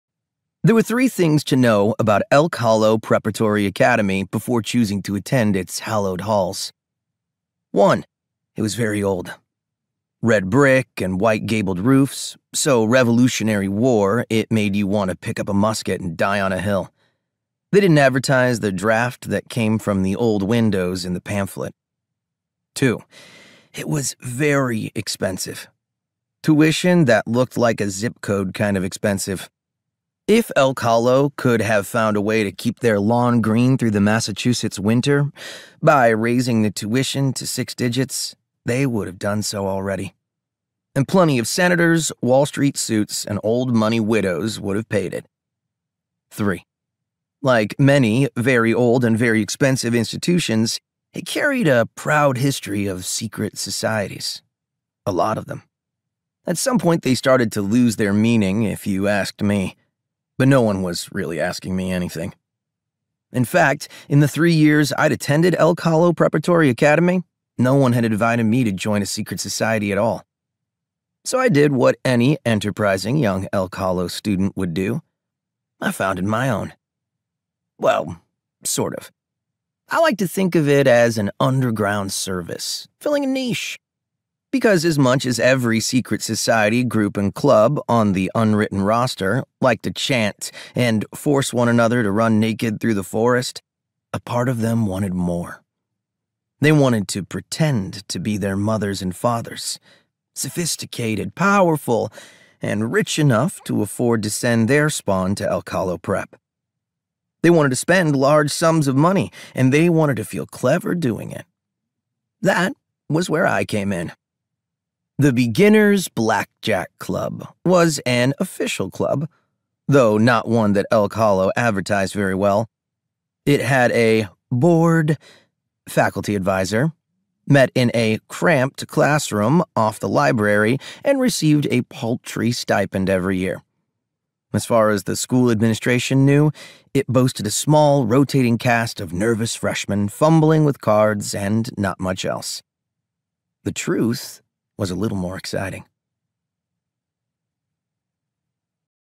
Audiobook Sample: Aces Wild
"Anyone can get lucky. But sometimes you need five aces to win."

Some people join chess club, some people play football. Jack Shannon runs a secret blackjack ring in his private school’s basement. What else is the son of a Las Vegas casino mogul supposed to do?

Everything starts falling apart when Jack’s mom is arrested for their family’s ties to organized crime. His sister Beth thinks this is the Shannon family’s chance to finally go straight, but Jack knows something’s not right. His mom was sold out,
and he knows by whom: Peter Carlevaro, rival casino owner and jilted lover.

Gross.

Jack hatches a plan to find out what Carlevaro’s holding over his mom’s head, but he can’t do it alone. He recruits his closest friends—the asexual support group he met through fandom forums. Now all he has to do is infiltrate a high-stakes
gambling club and dodge dark family secrets, while hopelessly navigating what it means to be in love while asexual. Easy, right?